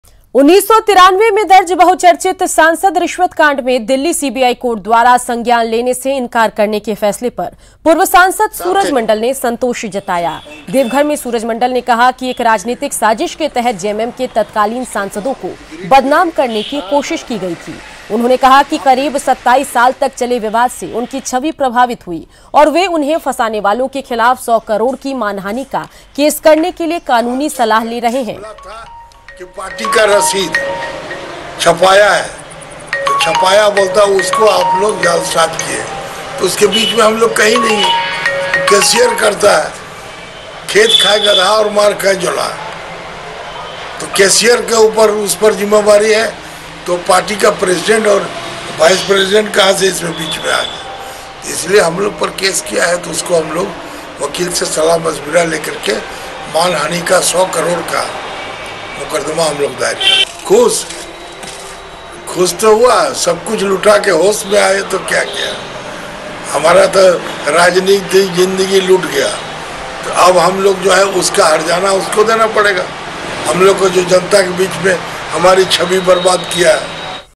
कोर्ट के फैसले से Suraj Mandal खुश, बताया साजिस के तहस बदमाश करने की थी कोशिश
बहुचर्चित रिश्वत कांड में आरोपी लोगों को राहत मिल गयी है पूर्व सांसद  Suraj Mandal फैसले को लेकर ख़ुशी जताई !